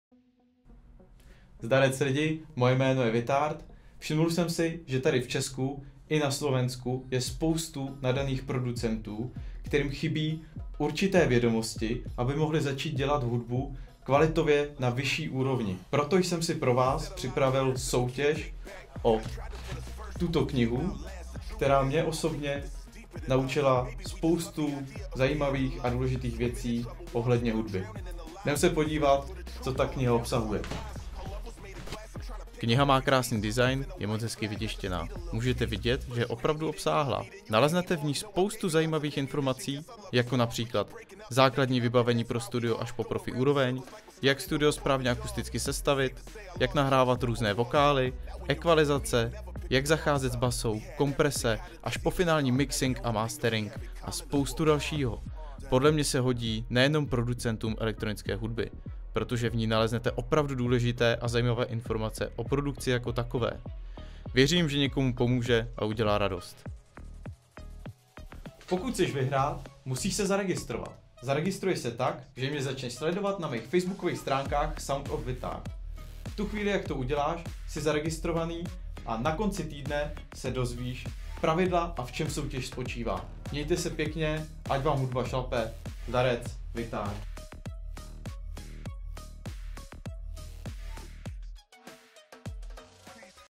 Soutěž o knihu Secrets of dance music production
Chceš vyhrát tuto knihu : Secrets of Dance Music Production?!🎧
Tak se zaregistruj. A výhra může být tvá.🥳
Kniha obsahuje, všechny potřebné informace od vybavení až po finální Mixing/Mastering. 🎶
Pravidla registrace : Začni mě sledovat na Facebooku. ) Na konci týdne se dozvíš pravidla a v čem soutěž spočívá.
Informační video nalezneš dole v komentářích 👇